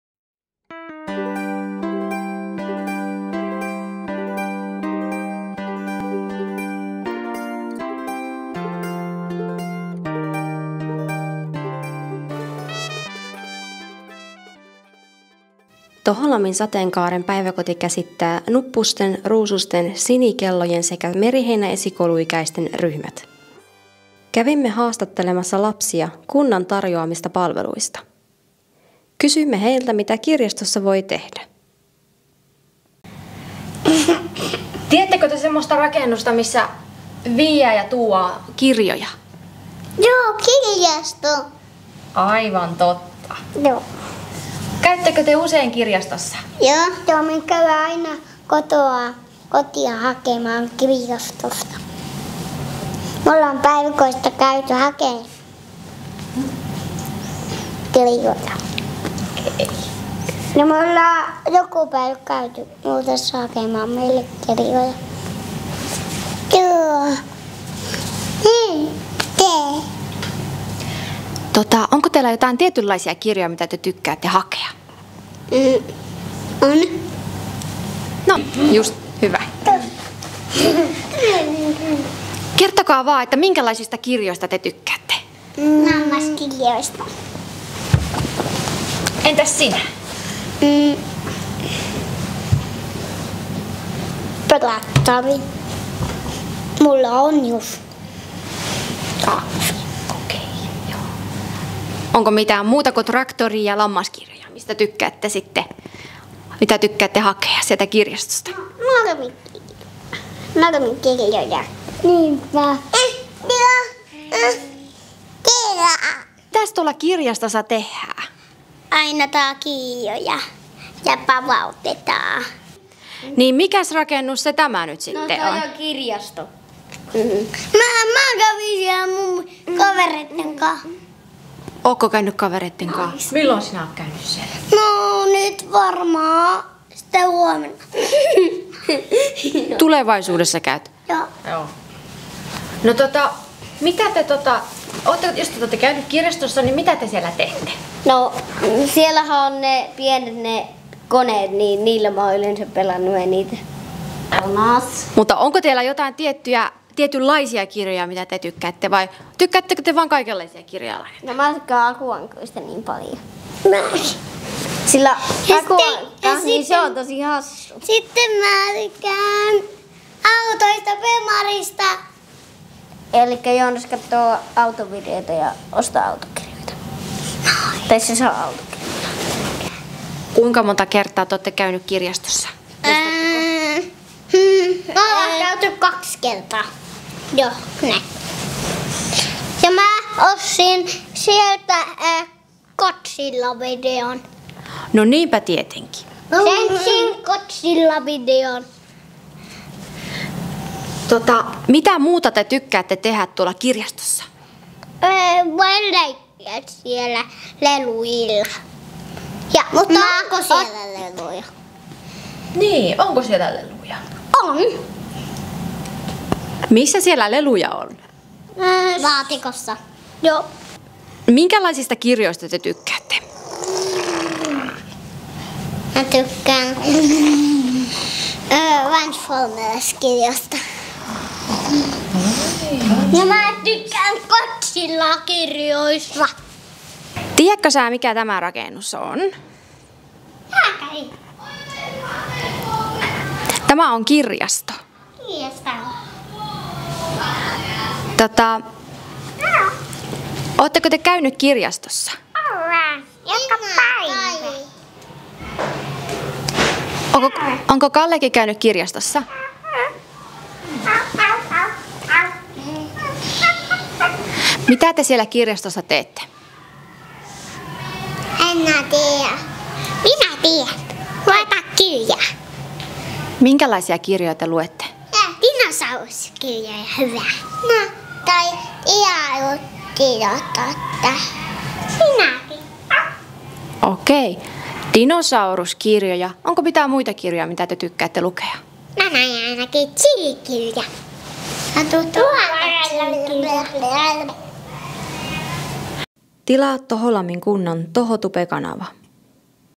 Luonnollisen kotilaakson kuulumisia. Lampin lapset kertovat kirjastosta
Tässä videossa lapset kertovat Toholammin kunnan kirjastosta